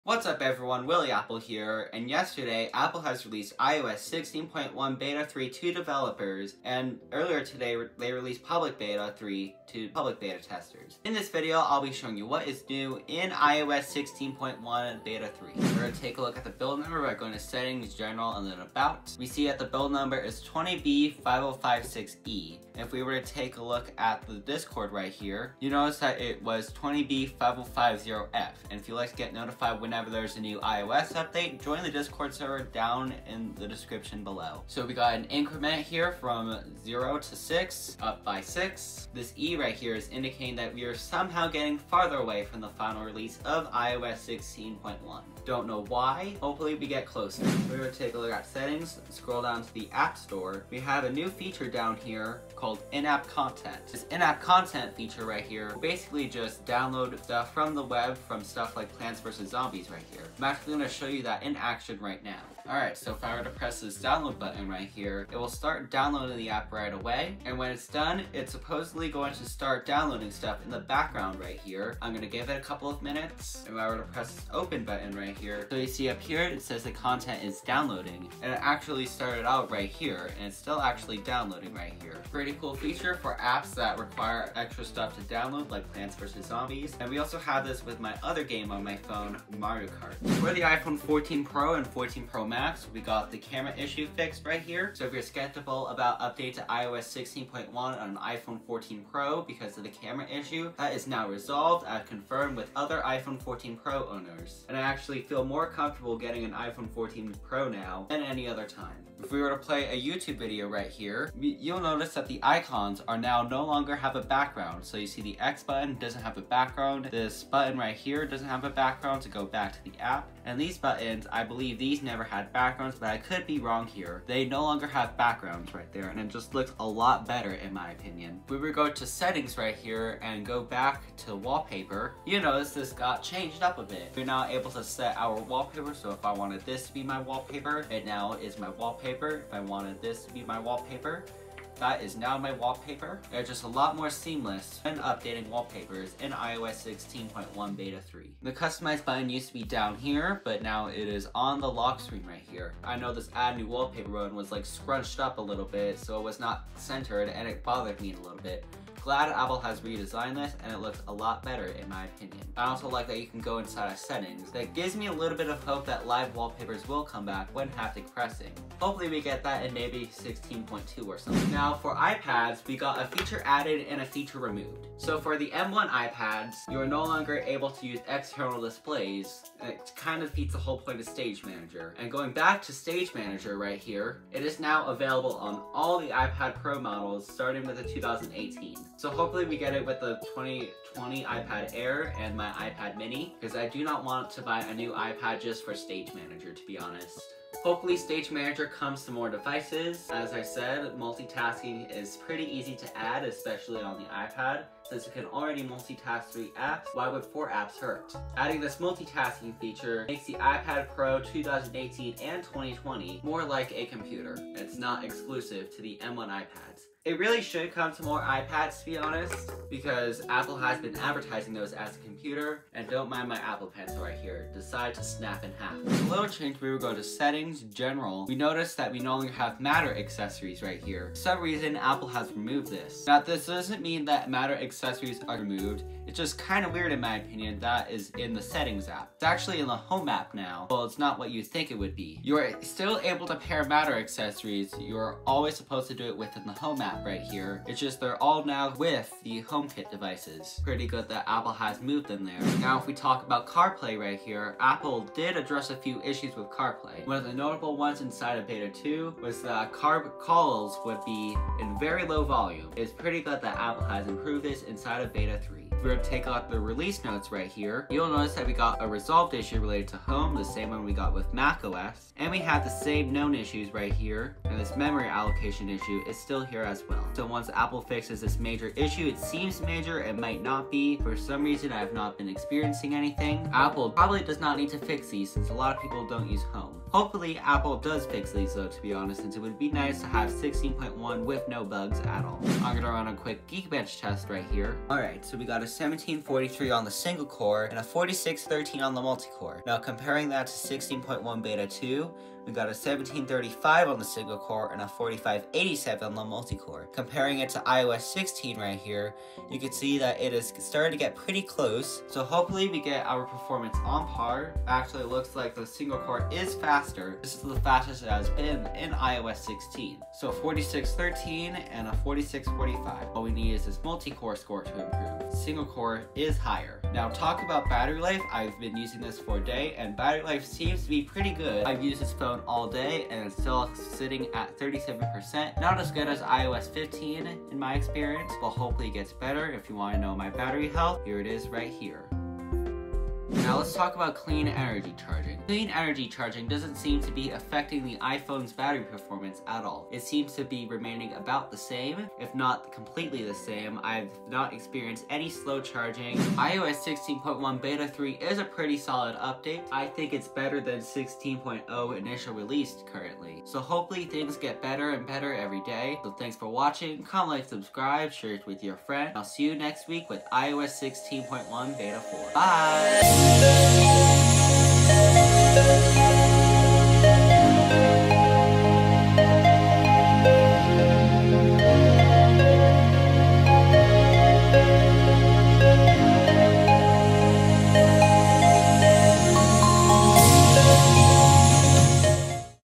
iOS 16.1 Beta 3 - What's new?
Apple has released iOS 16.1 Beta 3 to beta testers, and it has a bunch of new features. In this video, I will show you what is new in this software.